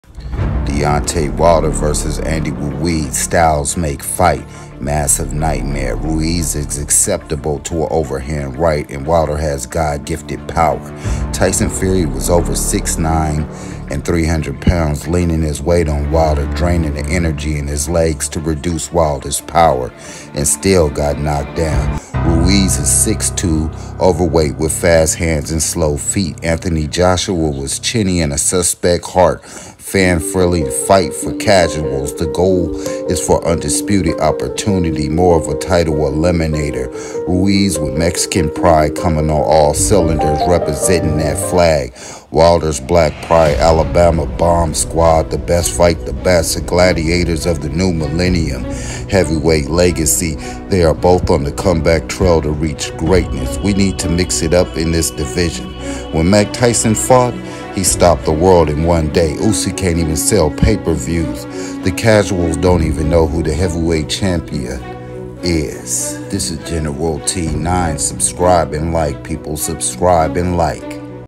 Deontay Wilder Can Knockout Andy Ruiz in this MEGA FIGHT 🤯
Deontay Wilder can knock out Andy Ruiz in this upcoming mega fight